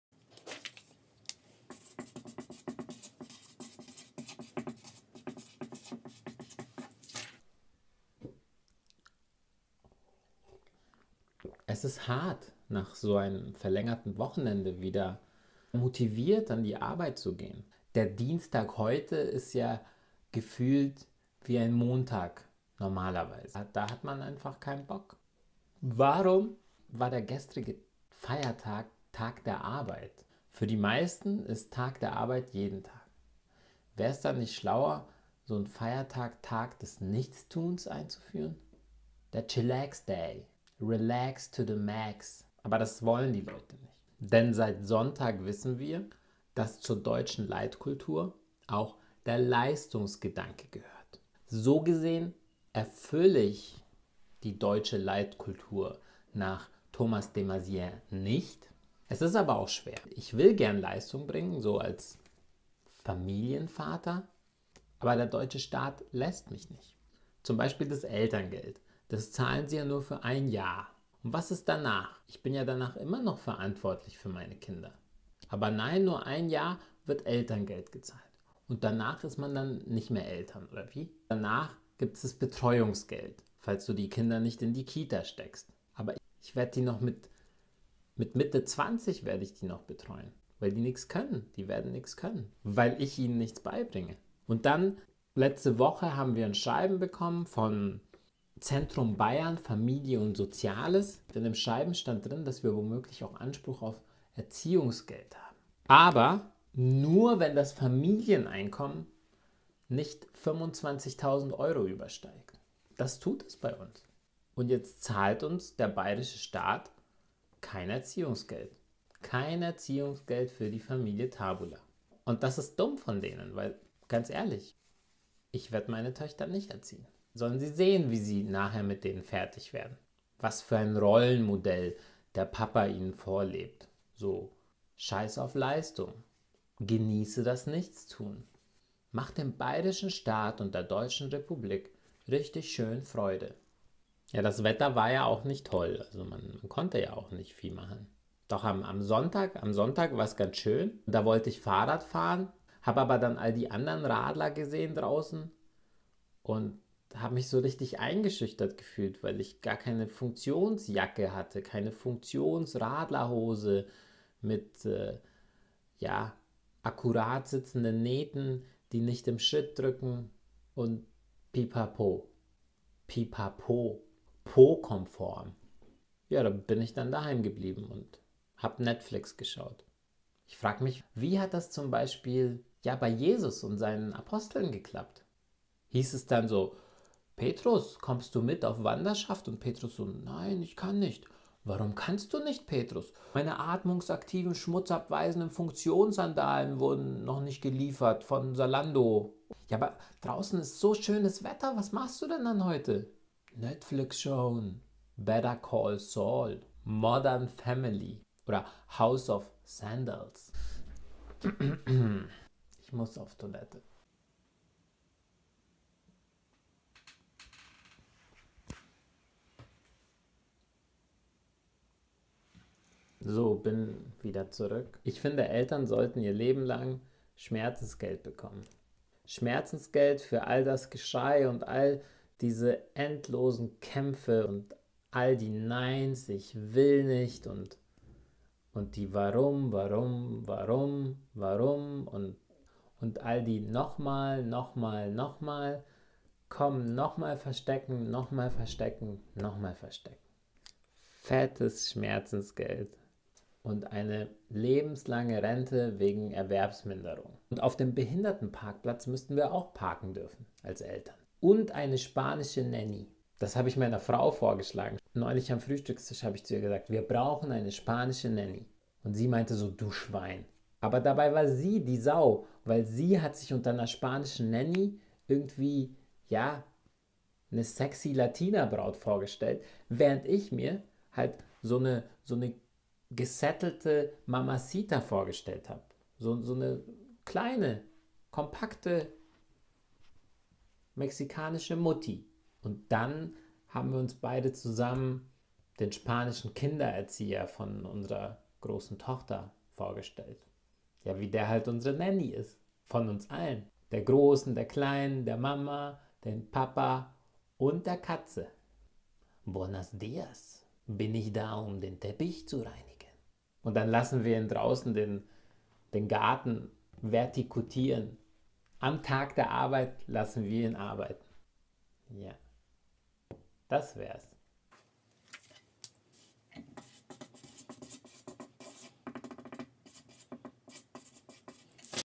Der Montag der Dienstage
Mates Morgen - Tag 45

Ein neuer Morgen, ein neues Video: u.a. mit Thomas De Maizière, Jesus, Petrus und ein wenig süße Miezekatze. Check it out!